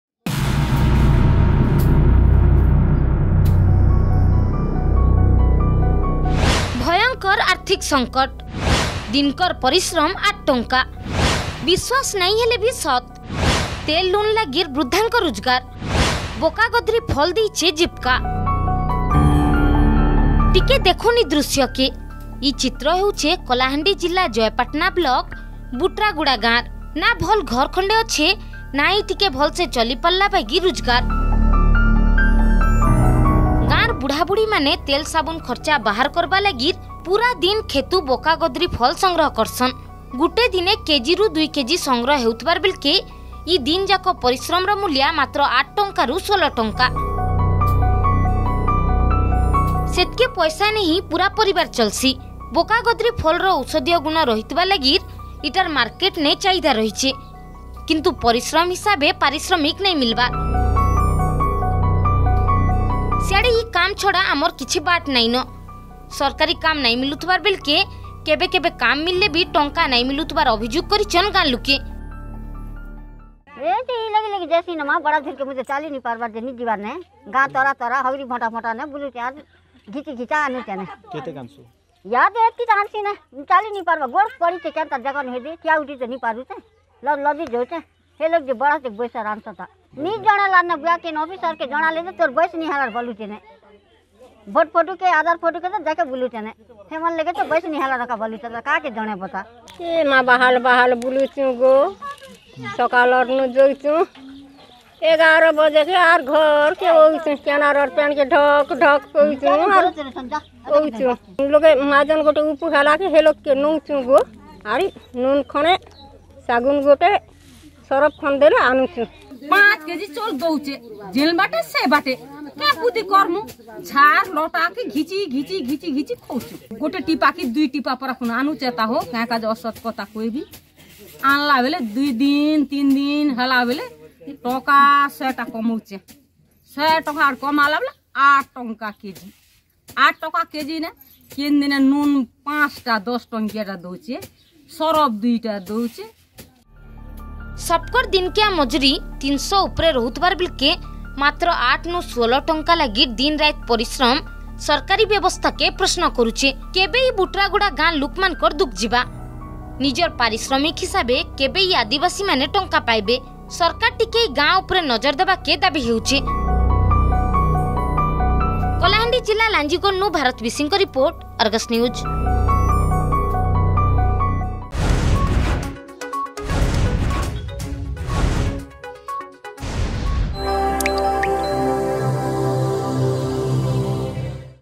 ଭୟକଁର୍ ଆର୍ଥିକ୍ ସଙ୍କଟ୍
ନାଁ ଭଲ୍ ଘର୍ ଖଣ୍ଡେ ଅଛେ ନା ହି ଟିକେ ଭଲସେ ଚଲି ପାରଲା ବାଗିର୍ ରୁଜଗାର୍ ।  ଗାଁର୍ _ ବୁଢା ବୁଢୀମାନେ ତେଲ୍ ସାବୁନ୍ ଖର୍ଚ୍ଚା ବାହାର୍ କରବାର୍ ଲାଗିର୍ ପୂରା ଦିନ୍ କ୍ଷେତୁ ବୋକାଗୋଦ୍ରୀ ଫଲ ବେଟୁଛନ୍ ।